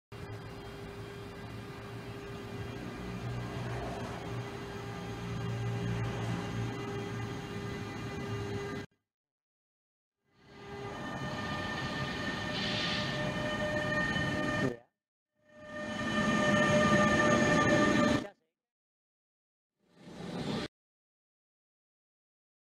Vertrek NS Traxx 186 236 te Zwolle - testrit SW9-software
NS Traxx 186 236 vertrekt los te station Zwolle. De locomotief voor wat testslagen over de Hanzelijn voor het testen van SW9-software